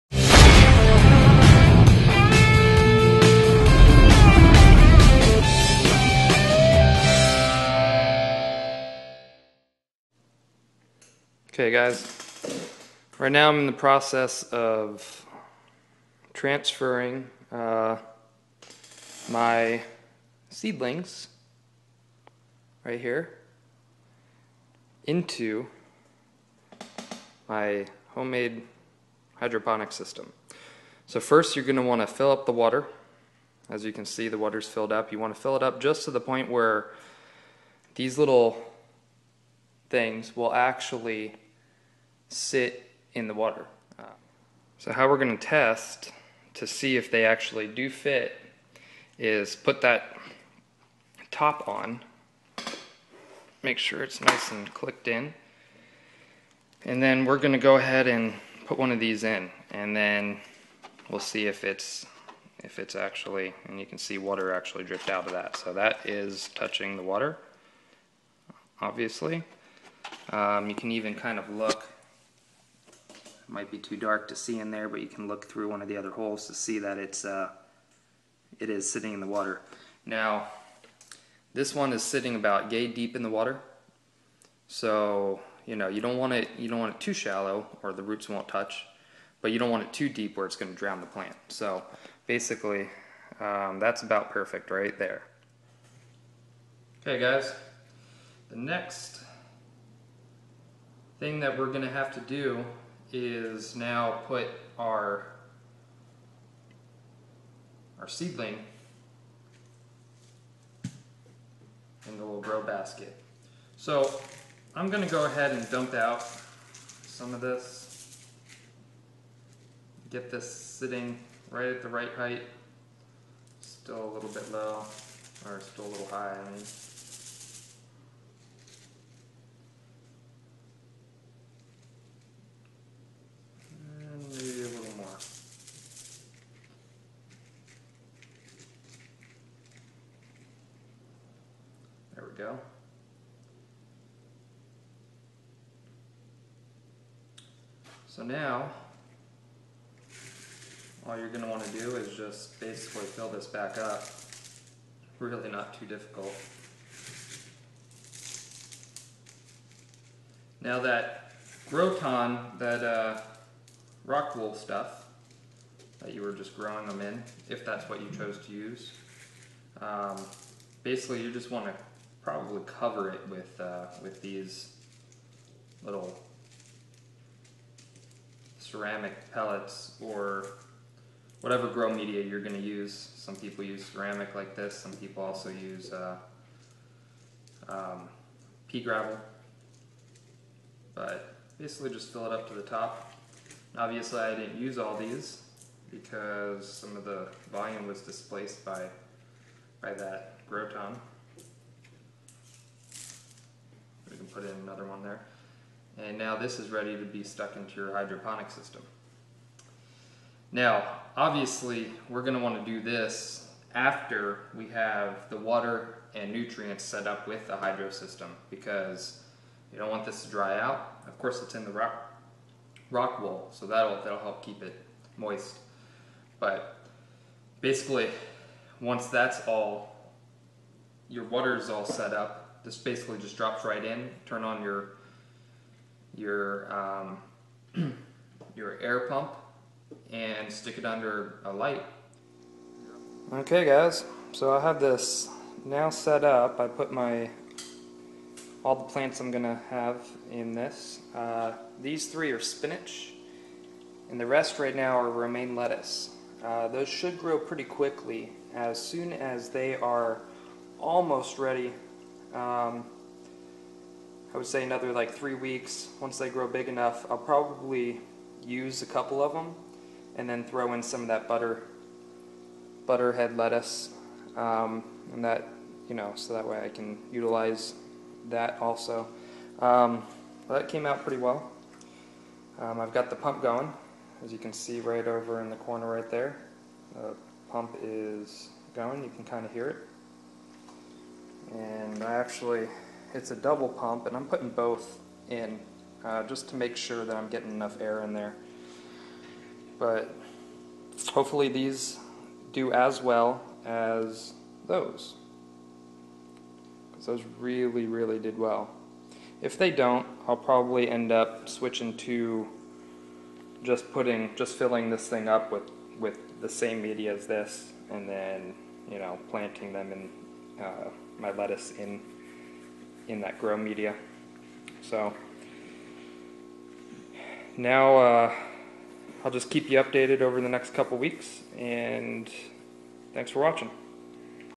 Transfering Seedlings from Rockwool to Hydroponic System
This is how I transferred my seedlings in rock wool, to my hydroponics system.